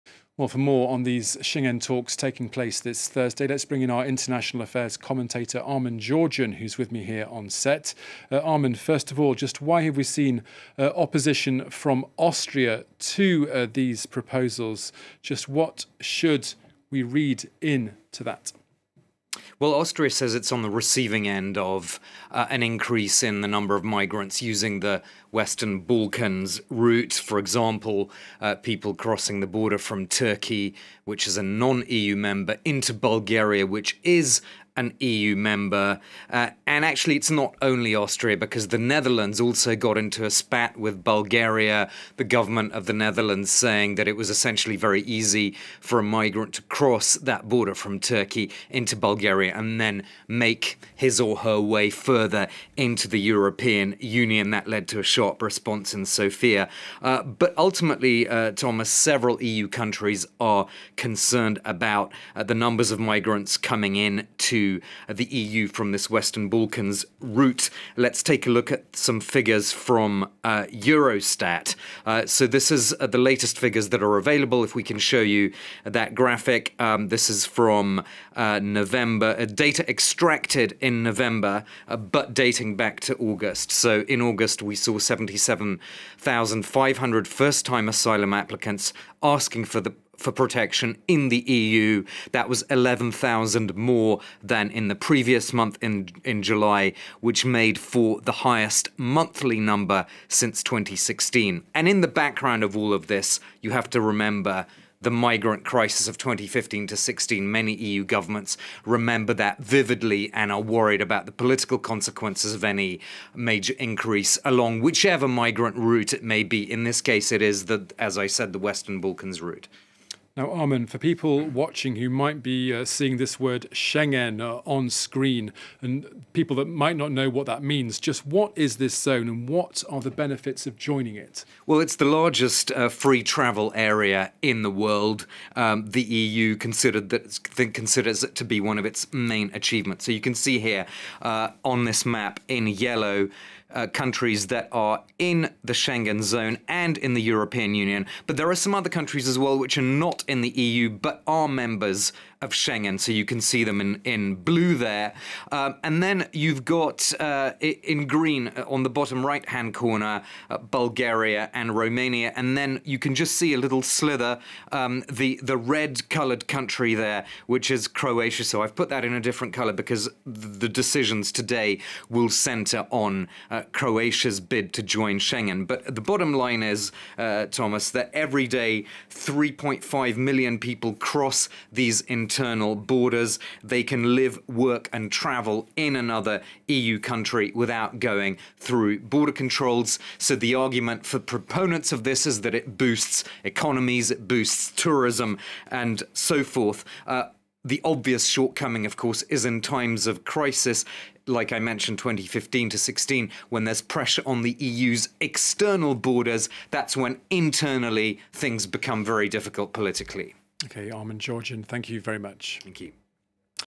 Austria to veto Bulgaria, Romania joining Schengen area • FRANCE 24 English
Austria on Thursday said it will veto the EU's two poorest members Romania and Bulgaria joining the bloc's Schengen visa-free zone, dashing their decade-long bid. Decisions on Schengen enlargement have to be taken unanimously. The EU ministers were expected to approve Croatia joining Schengen, which would expand the area to include 23 of the EU's 27 member states, plus Switzerland, Norway, Liechtenstein and Iceland. FRANCE 24's International Affairs Editor Armen Georgian tells us more.